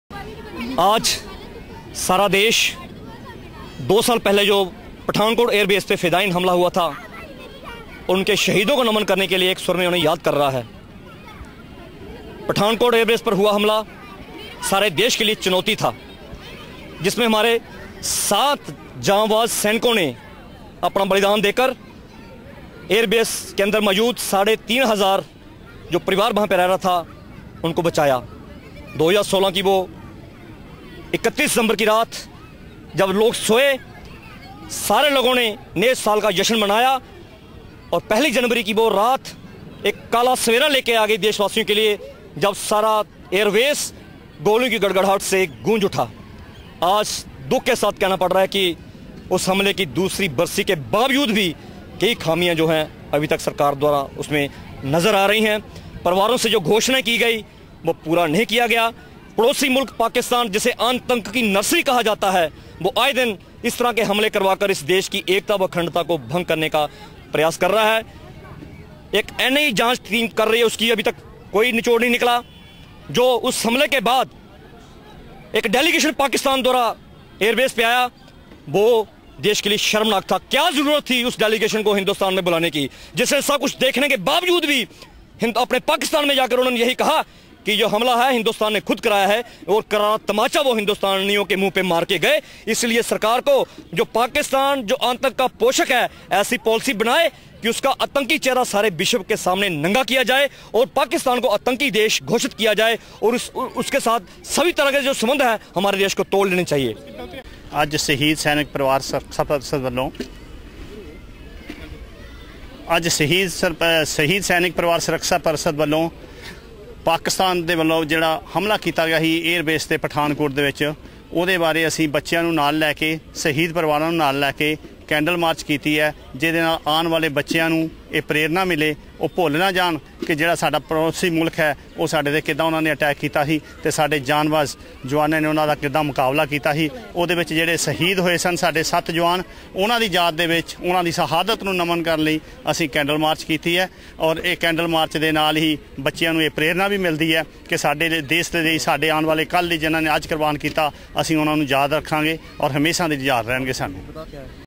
Candlelight march for soldiers killed in Pathankot attack
जिला पठानकोट के एयरबेस स्टेशन पर 2 जनवरी 2016 की सुबह हुए आतंकी हमले की बरसी के चलते आज स्थानीय लोगो और शहीद सैनिकों के परिवारों द्वारा कैंडल मार्च निकाला गया जिसमें स्कूली बच्चों ने बड़ी गिनती में हिस्सा लिया इस मोके शहीद सैनिक सुरक्षा परिषद् के प्रधान ने कहा की आज पठानकोट एयरबेस स्टेशन पर हुए आतंकी हमले के चलते हमारी तरफ से कैंडल मार्च निकला गया है जिसमें स्कूली बच्चों के करीब 30 शहीद सैनिकों के परिवारों ने शामिल हो शहीदों को श्रदांजली दी  उन्होंने कहा उस आतंकी हमले याद आज शहीद सैनिकों के परिवारों के साइन में आग की तरह जल रही है उन्होंने कहा की सरकार की तरफ से उस समय शहीदों के परिवार से जो वादे किये गए थे वो अभी तक ब्फ नहीं हो पाए है और आतंक की उस रात के बाद पाकिस्तान का एक बफ़द भारत आया था जिसने जहाँ आकर जाँच की पाकिस्तान में वापिस जा ये कहा की भारत ने ये हमला खुद करवाया जोकि भारत के लिए शर्म की बात है इस लिए भारत सरकार को चाहिए की पाकिस्तान के साथ सभी सबंध तोड़ उसे मुँह तोड़ जवाव दे